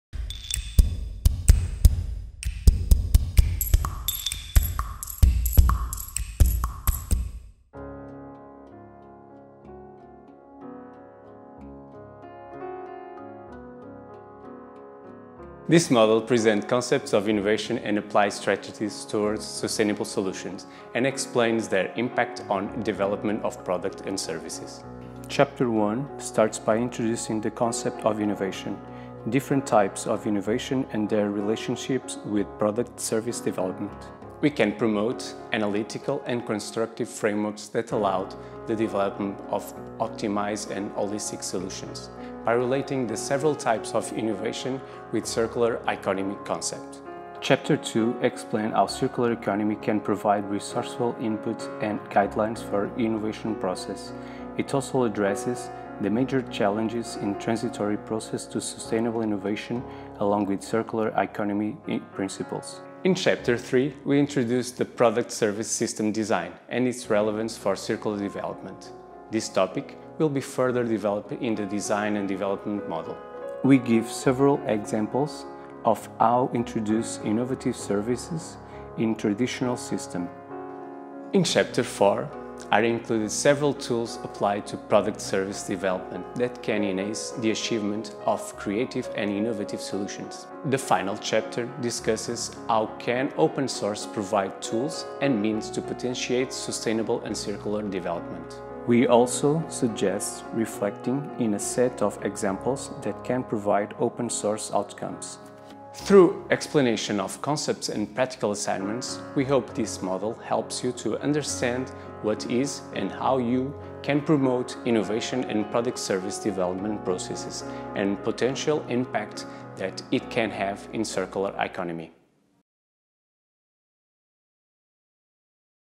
KATCH e Video Radical Innovation and Collavorative Design Processes
Considering the impacts of the design activity on industry and society at large, how can radical innovation change behaviours and mind-sets, specially related to sustainable consumption and environmental education?
This question will be answered in this module, among many others.